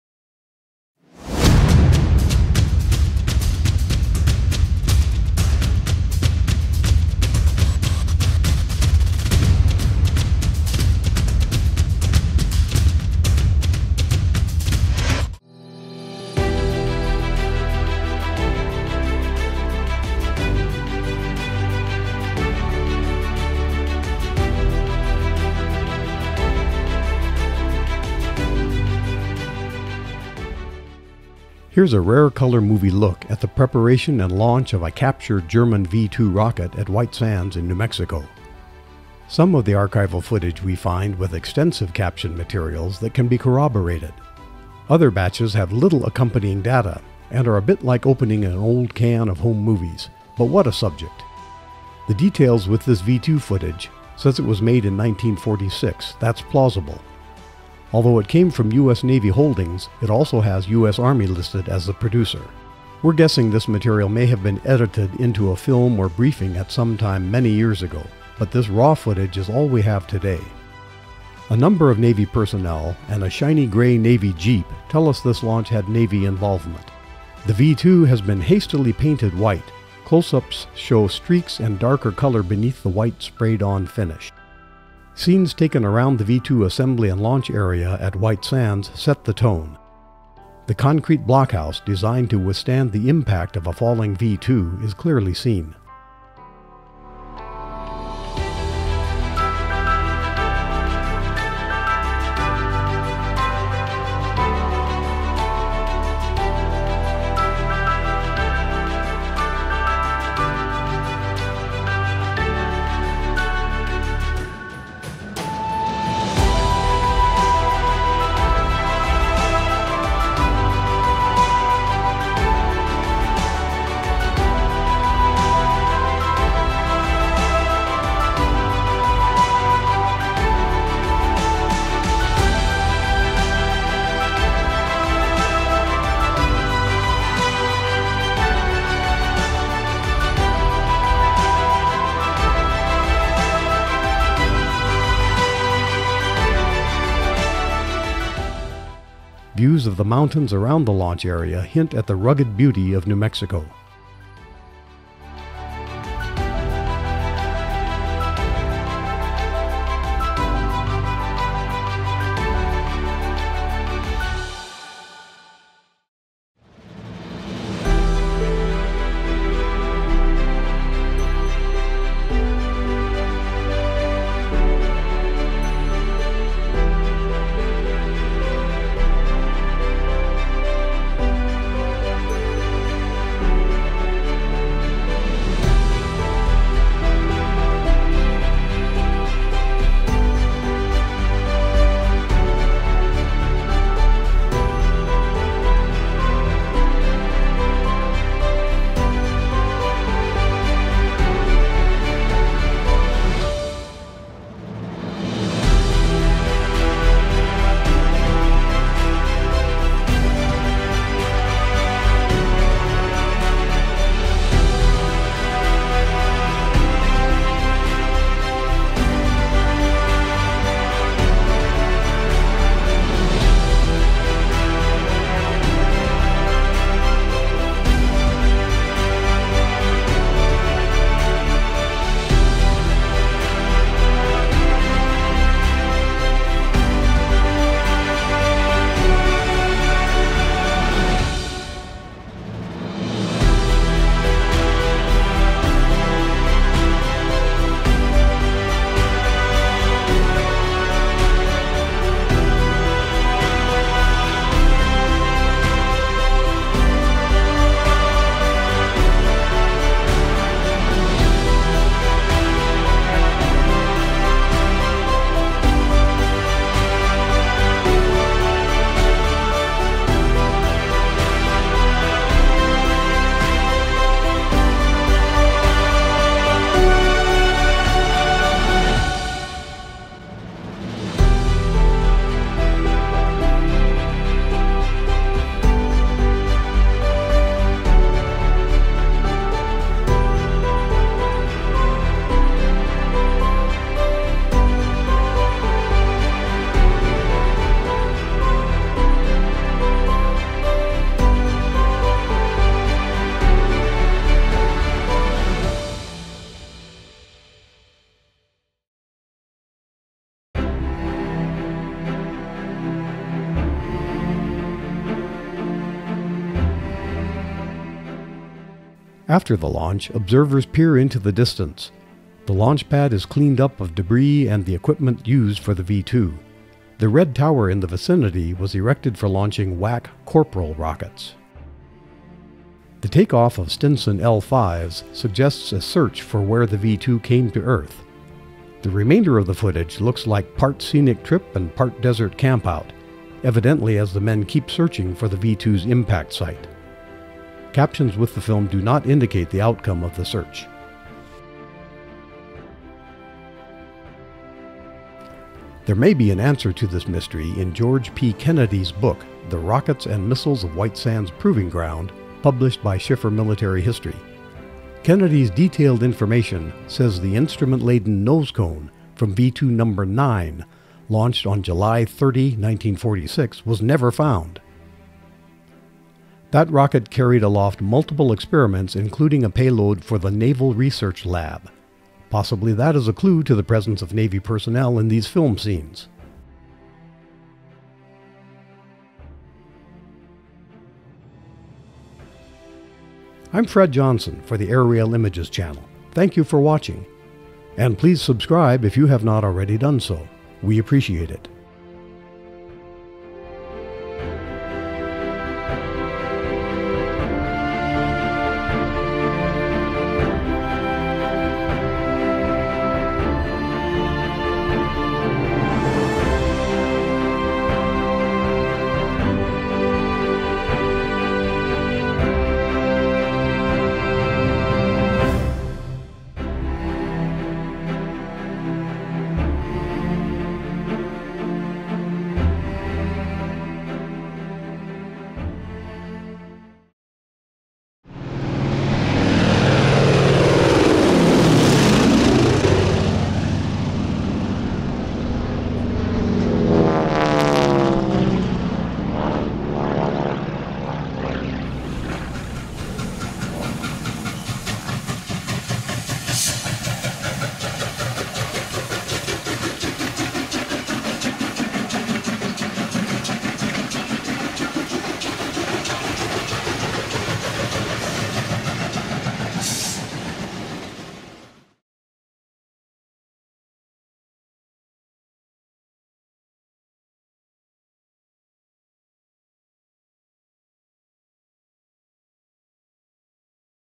Color V-2 Launch in New Mexico - Alamogordo White Sands
Here's a rare color movie look at the preparation and launch of a captured German V-2 rocket at White Sands in New Mexico.
 Some of the archival footage we find with extensive caption materials that can be corroborated; Other batches have little accompanying data, and are a bit like opening an old can of home movies  -- but what a subject!
 The details with this V-2 footage says it was made in 1946; that's plausible. Although it came from U.S. Navy holdings, it also has U.S. Army listed as the producer.
 We're guessing this material may have been edited into a film or briefing at some time many years ago, but this raw footage is all we have today. A number of Navy personnel and a shiny gray Navy jeep tell us this launch had Navy involvement. The V-2 has been hastily painted white. Close-ups show streaks and darker color beneath the white sprayed-on finish.
 Scenes taken around the V-2 assembly and launch area at White Sands set the tone. The concrete blockhouse, designed to withstand the impact of a falling V-2, is clearly seen. Views of the mountains around the launch area hint at the rugged beauty of New Mexico.
 After the launch, observers peer into the distance. The launch pad is cleaned up of debris and the equipment used for the V-2. The red tower in the vicinity was erected for launching WAC Corporal rockets.
 The takeoff of Stinson L-5s suggests a search for where the V-2 came to earth. The remainder of the footage looks like part scenic trip and part desert campout, evidently as the men keep searching for the V-2's impact site. Captions with the film do not indicate the outcome of the search.
 There may be an answer to this mystery in George P. Kennedy's book, "The Rockets and Missiles of White Sands Proving Ground", published by Schiffer Military History. Kennedy's detailed information says the instrument-laden nose cone from V-2 Number 9, launched on July 30, 1946, was never found. That rocket carried aloft multiple experiments including a payload for the Naval Research Lab. Possibly that is a clue to the presence of Navy personnel in these film scenes.